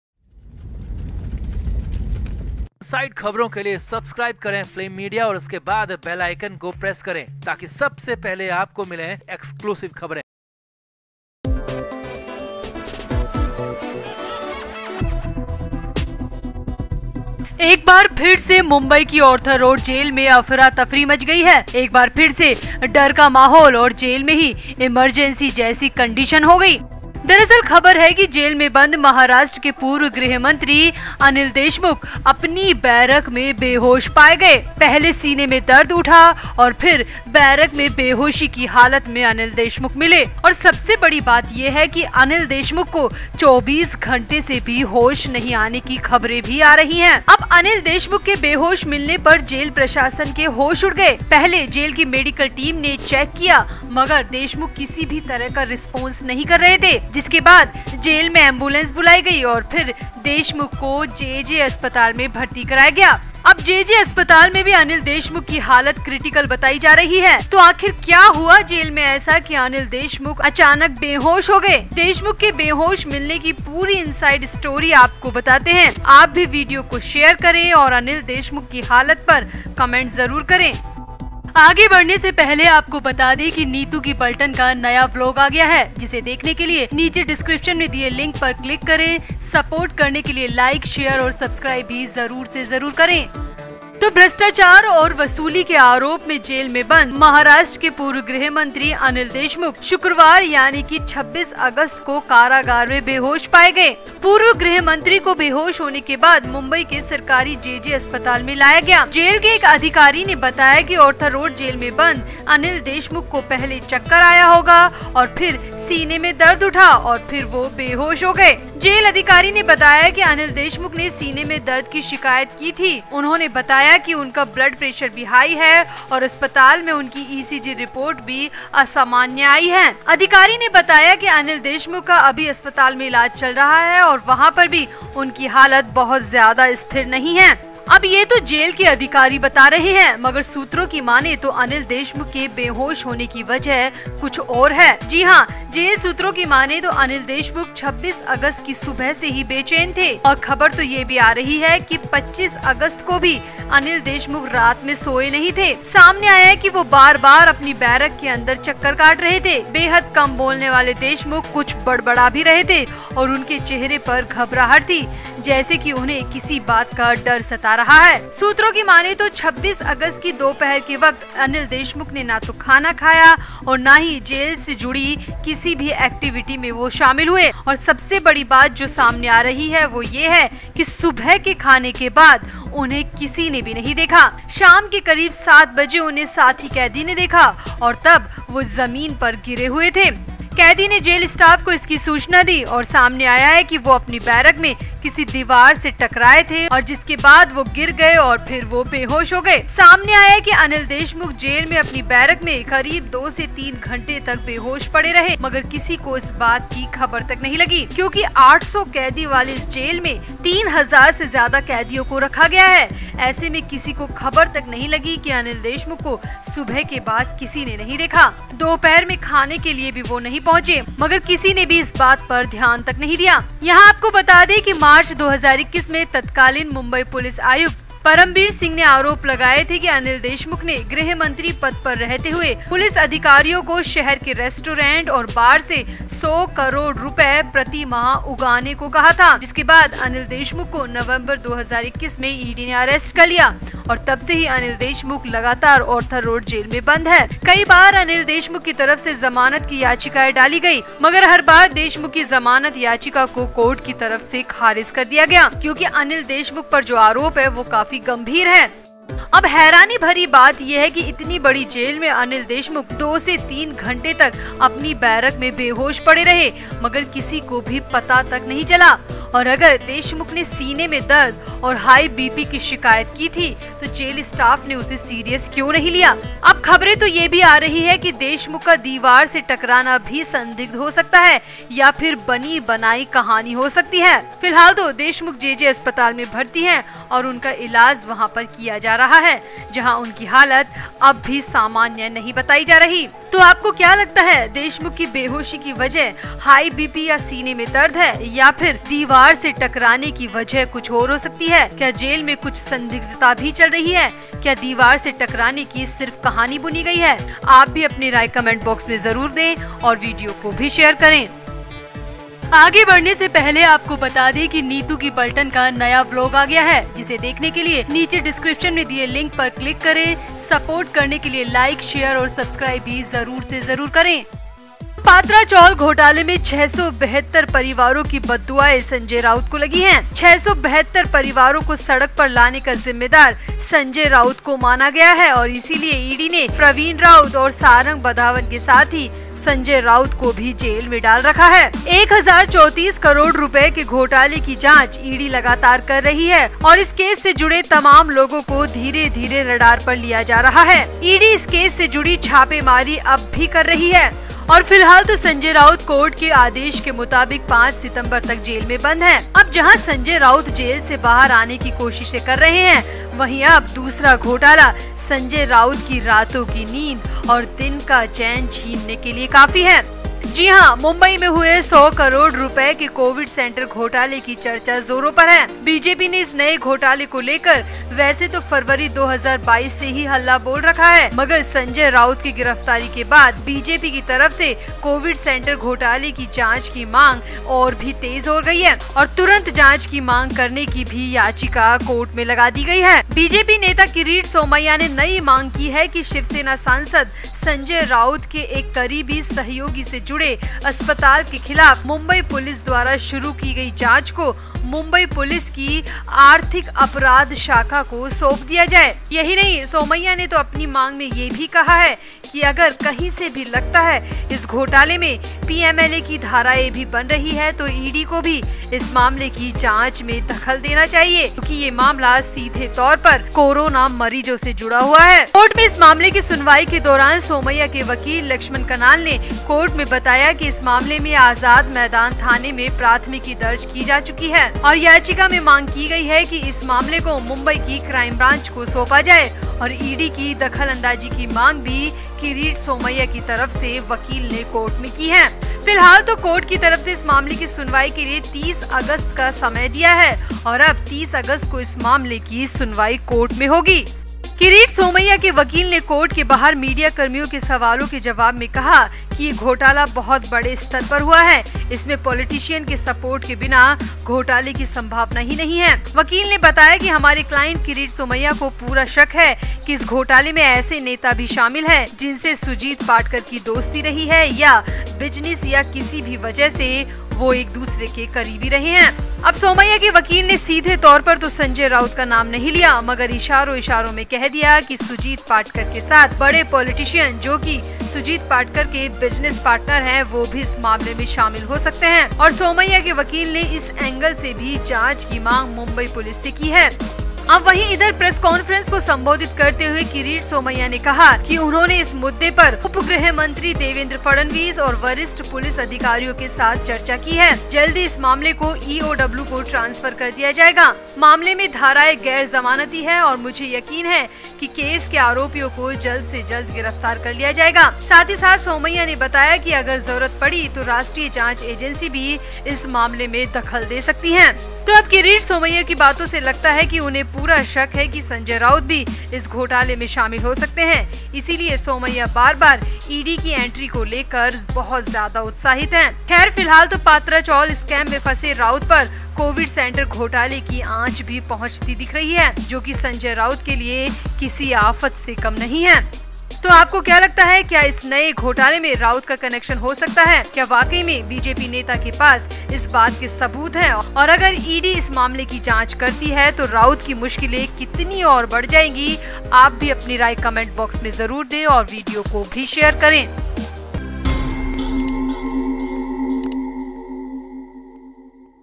दीवार से टकराकर बेहोश हुए अनिल देशमुख..जेजे अस्पताल में करवाया भर्ती..ऑर्थर जेल में मची अफरा तफरी
हमारा नया Vlog channel यहां देखें- 👇🏻👇🏻👇🏻👇🏻👇🏻